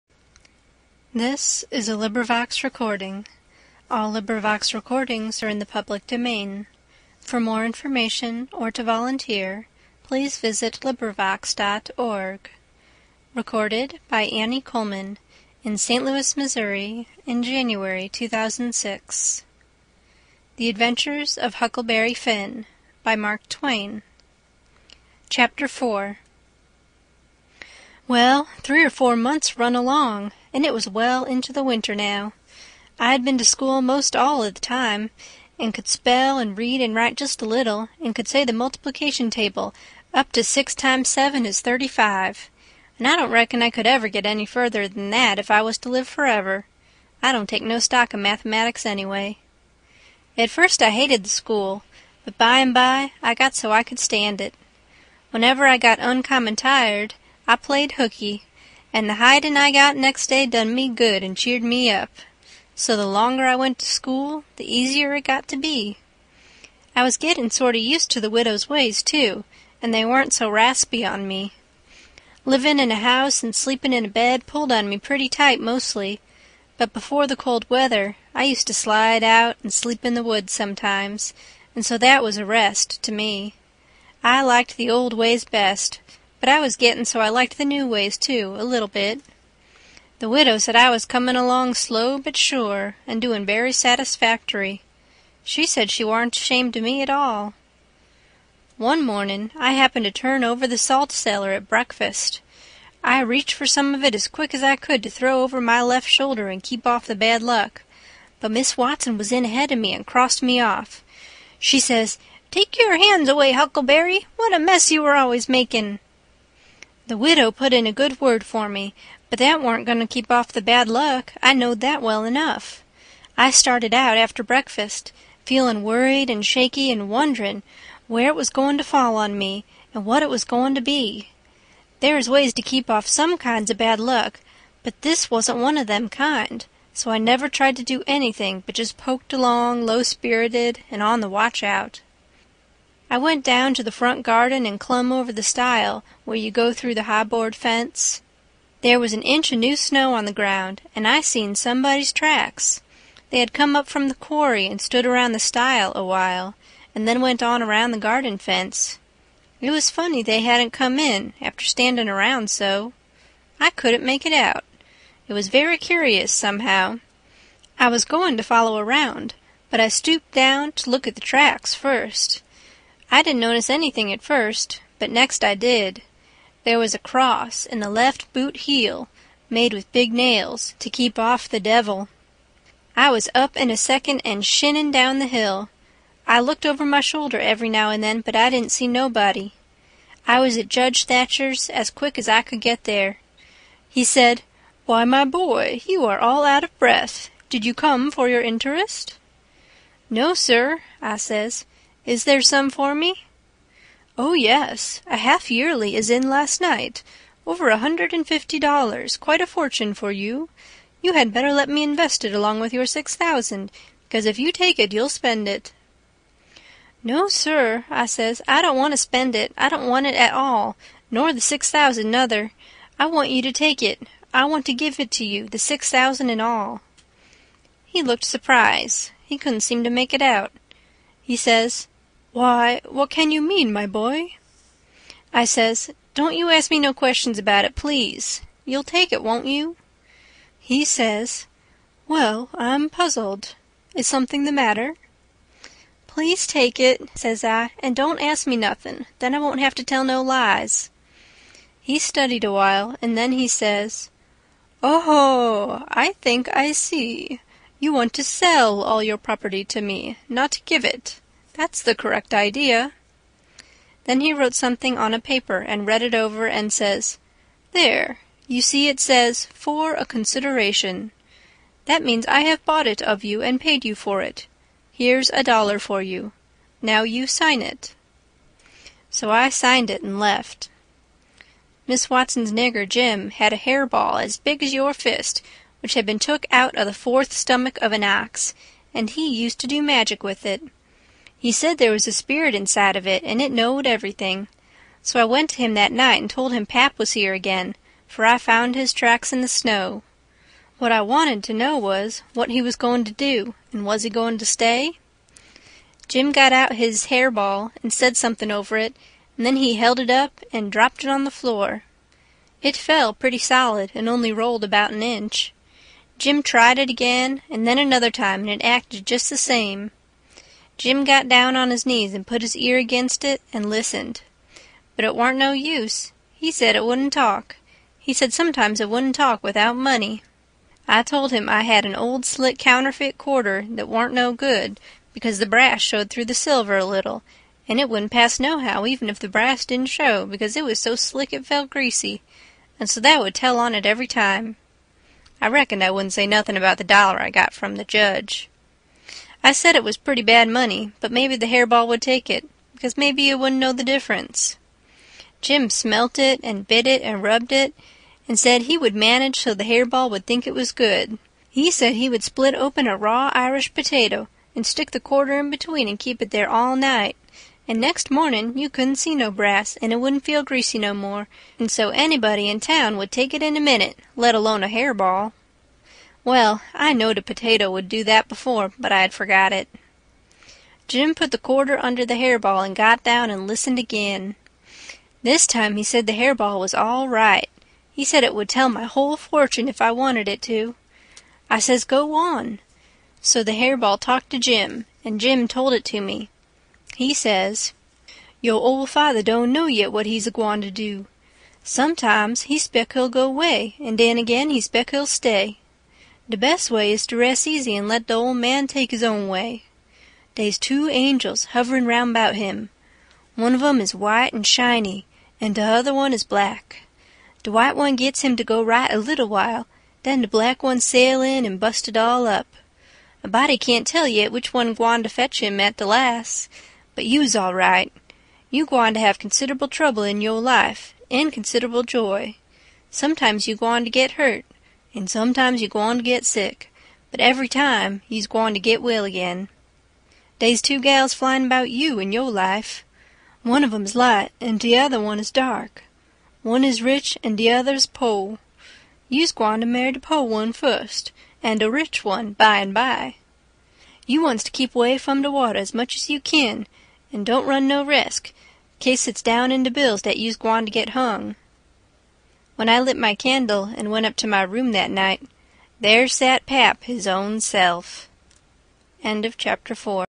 Adventures of Huckleberry Finn | Part - 4
Librivox recording of Adventures of Huckleberry Finn, by Mark Twain. Read by Annie Coleman. Adventures of Huckleberry Finn (1884) by Mark Twain is one of the truly great American novels, beloved by children, adults, and literary critics alike. The book tells the story of "Huck" Finn (first introduced as Tom Sawyer's sidekick in The Adventures of Tom Sawyer), his friend Jim, and their journey down the Mississippi River on a raft. Both are on the run, Huck from his drunk and abusive father, and Jim as a runaway slave.

As Huck and Jim drift down the river, they meet many colorful characters and have many great adventures. The true heart of the story, however, is the friendship between Huck and Jim. A constant theme throughout the book is Huck's internal struggle between what he has been taught, that helping a runaway slave is a sin, and what he truly believes, that Jim is a good man and it couldn't possibly be wrong to help him.